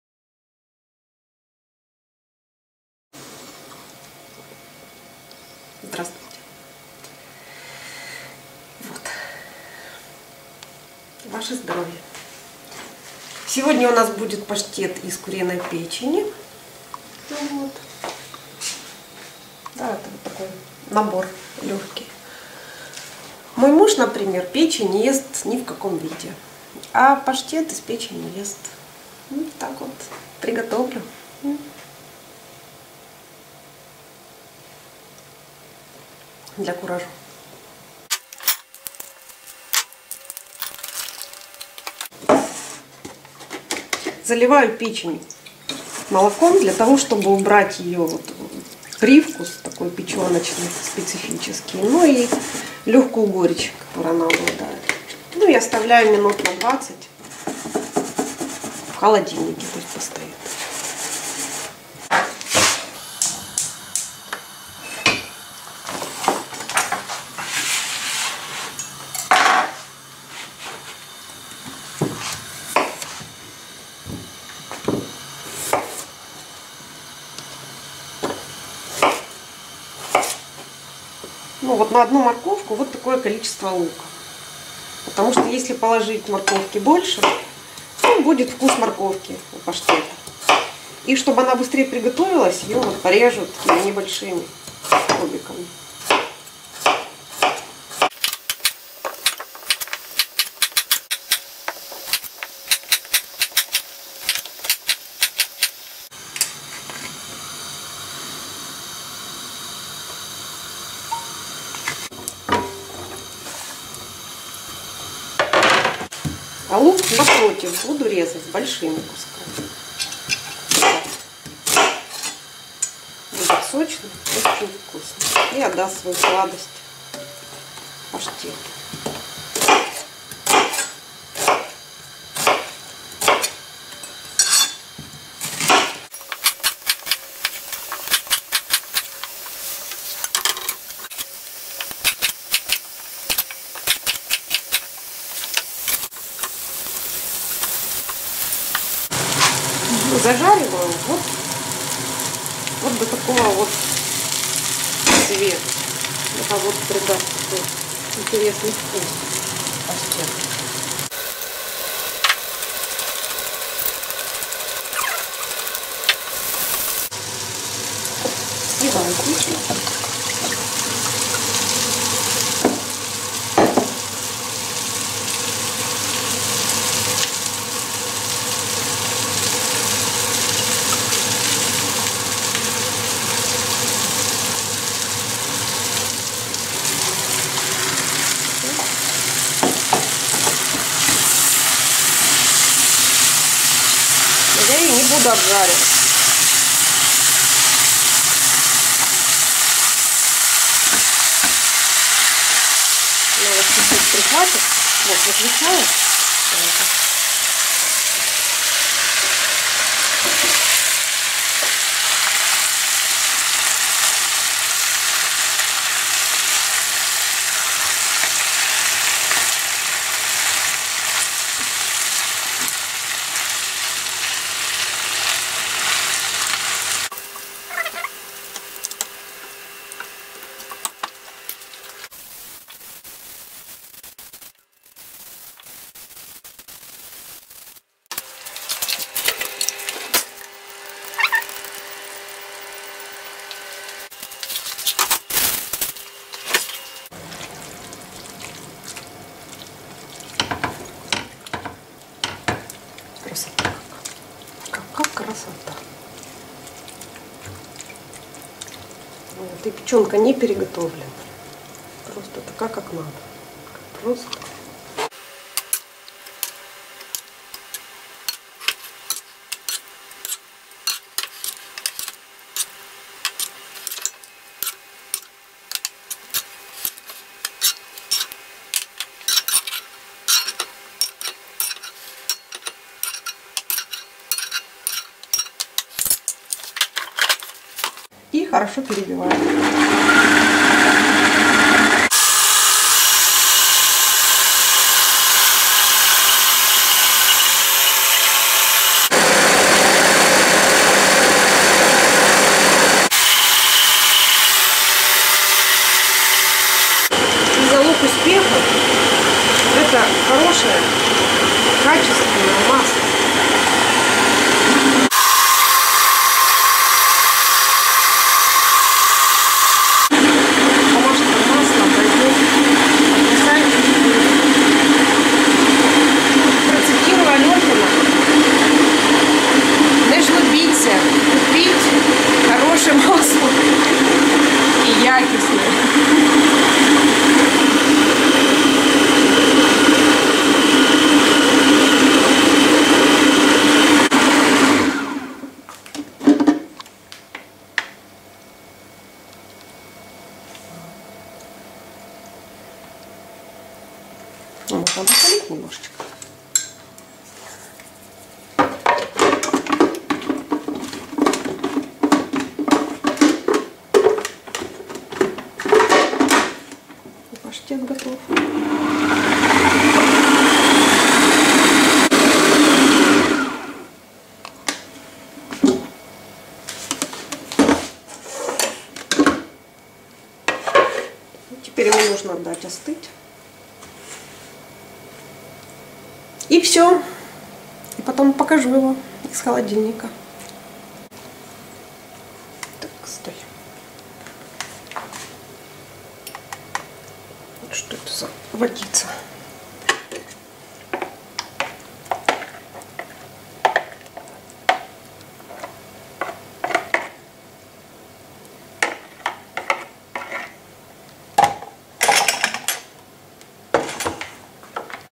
Паштет із курячої печінки - Chicken liver pate
Паштет - легка у приготуванні, але вишукана та смачна закуска. Мої паштети дуже популярні у родині та серед друзів, тому я вирішила записати відео приготування одного з різновидів - паштету з курячої печінки.
Pate (or rather "pâté") is easy to prepare, but delicious and tasty snack. My pate is very popular with family and friends, so I decided to record a video of cooking one of the varieties - chicken liver pate.